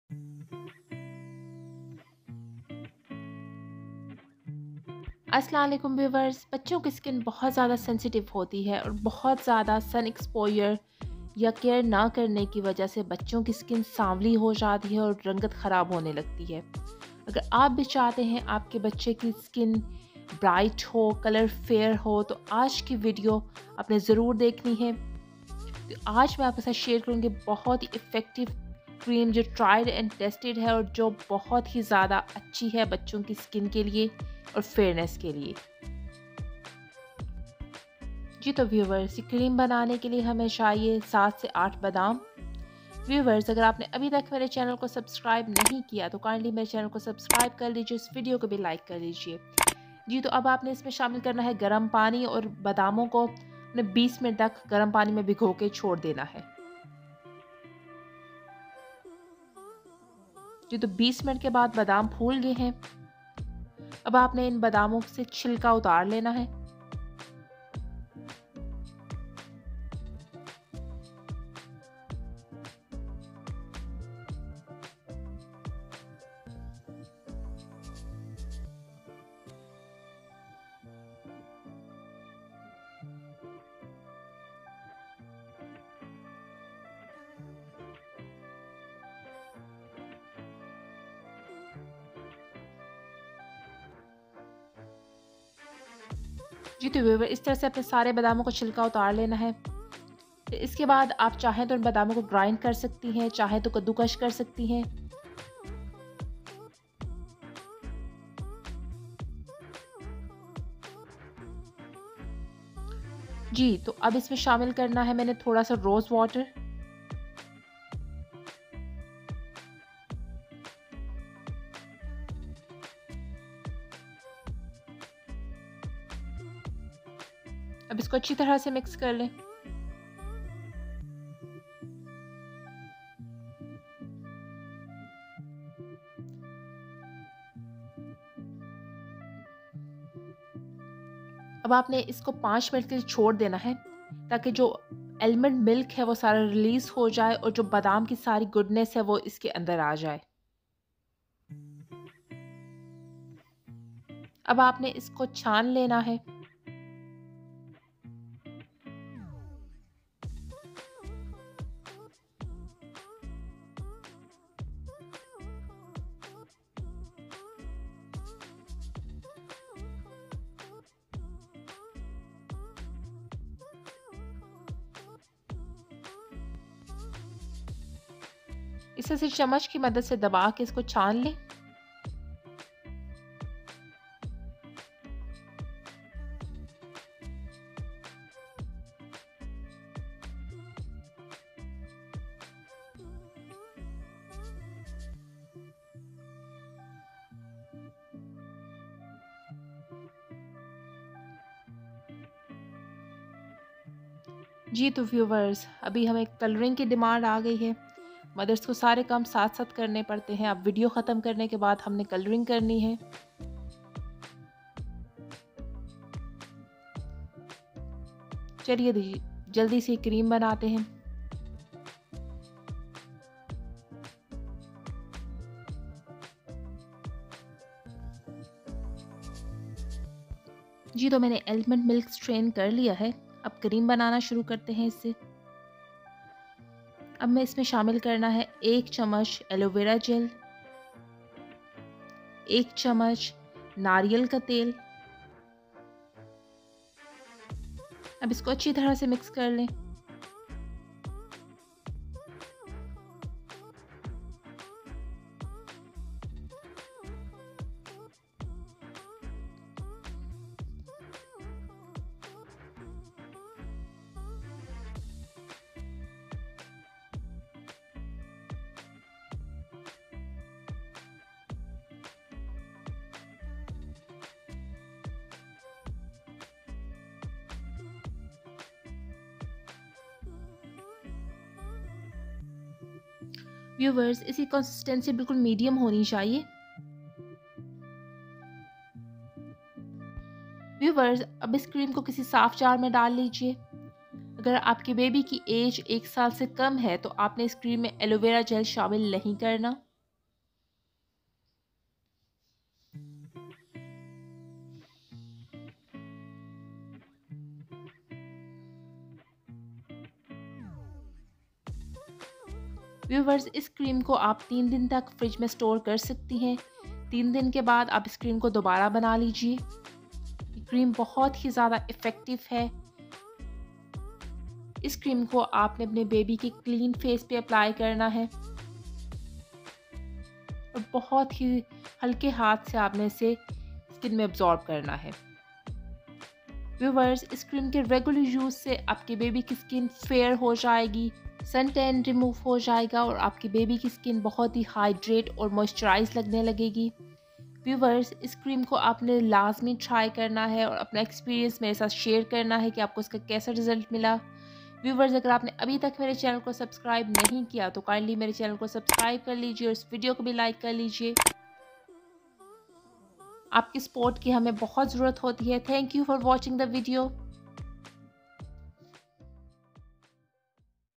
Baby Skin care remedy/children skin whitening natural cream/Bachon ka rang Gora Karne ka Tarika
Children skin is really sensitive and we should use only natural products to take care of baby's skin.
Here i shared a really effective cream that will make your child skin bright and fair naturally.